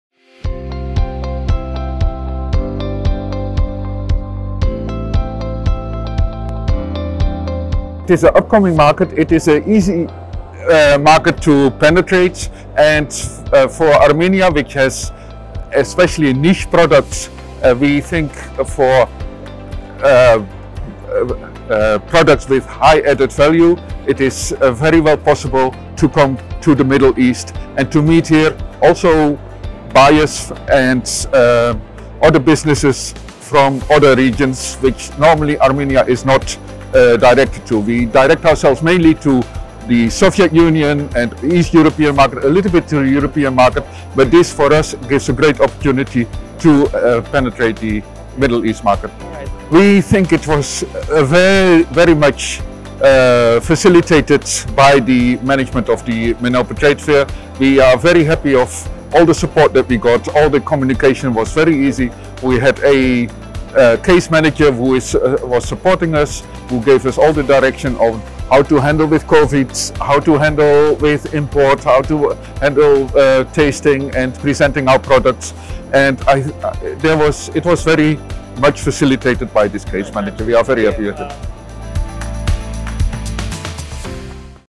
Bokkers Pascal, Team Leader, The Green Agriculture initiative Project in Armenia
Bokkers Pascal talks about his experience at the Organic and Natural Products Expo 2021.